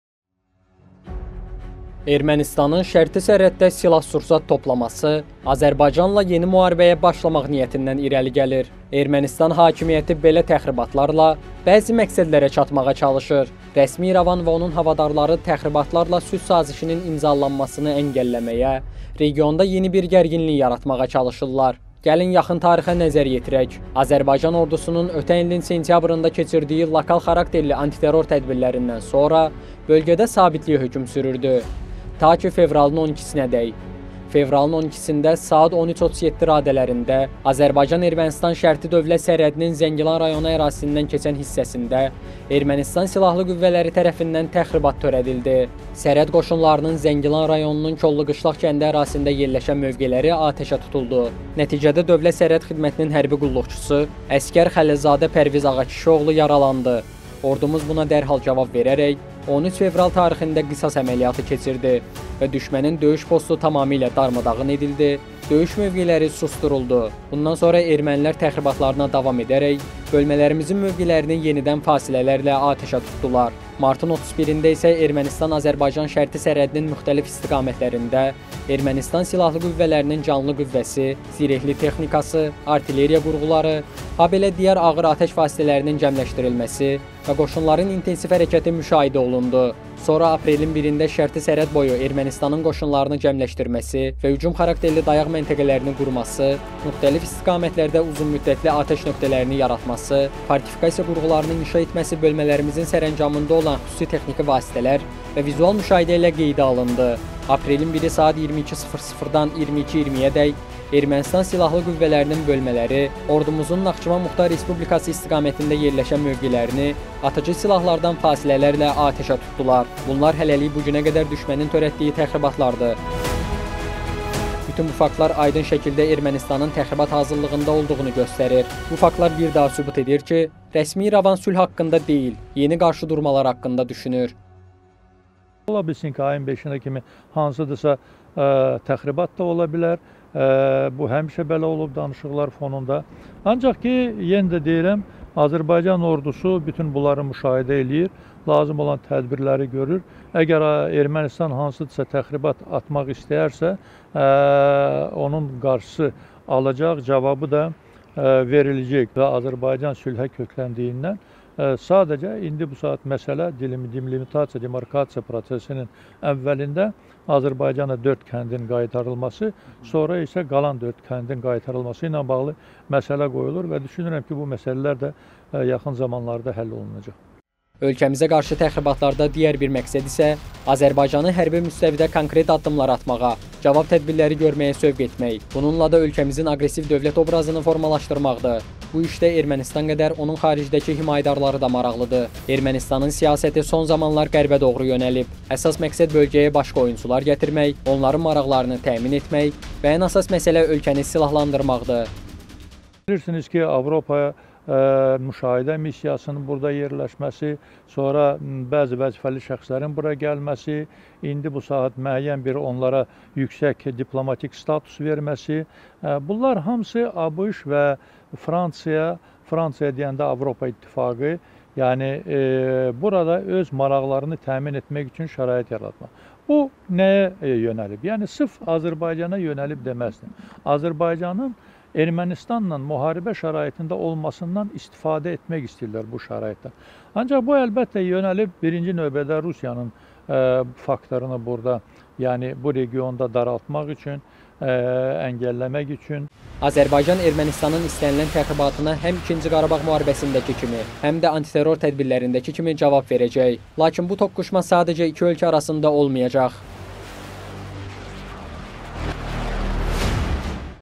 Şərti sərhəddə gərginlik davam edir
Ermənistan Qərbi bölgəyə cəlb etməklə yeni müharibəyə hazırlaşır